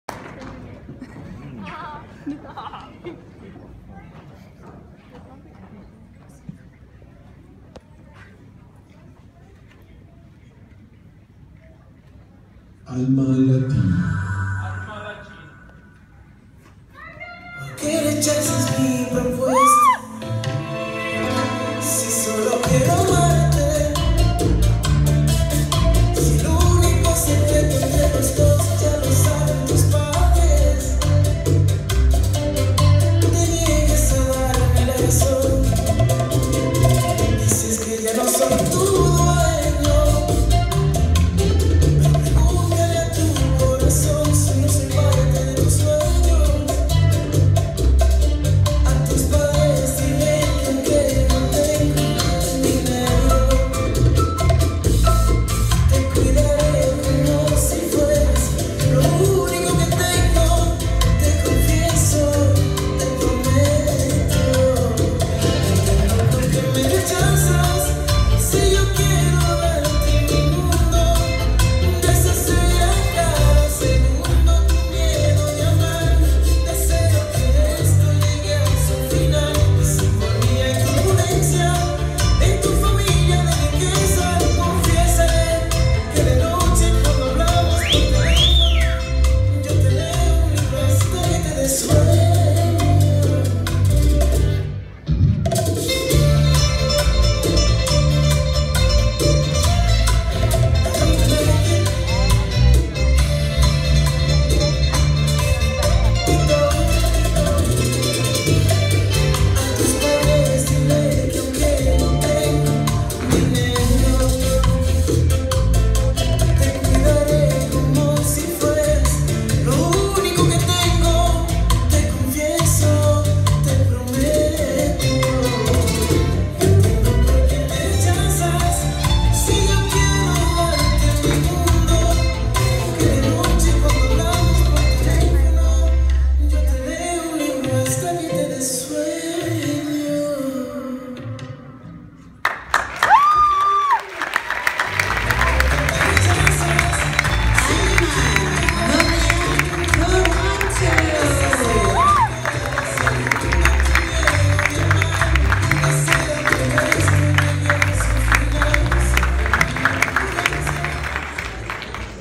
Alma Toronto & Alma Montreal Bachata at Toronto Salsa Festival 2016
Alma Toronto Season 5 Intermediate Bachata in collaboration with Alma Montreal at Toronto Salsa Festival 2016